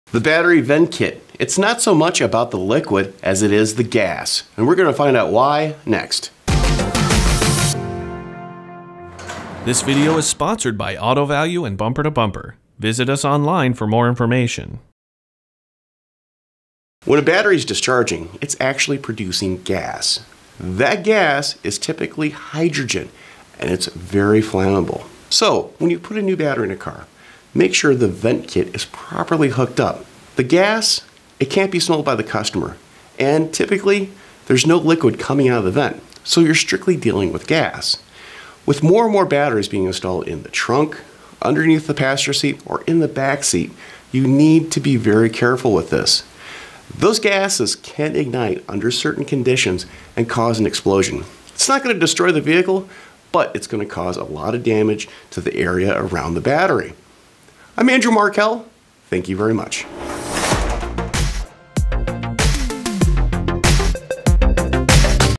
Battery vent kits | Maintenance Minute
Andrew Markel discusses battery vent kits, and why installing them properly is essential to ensure proper ventilation of gases.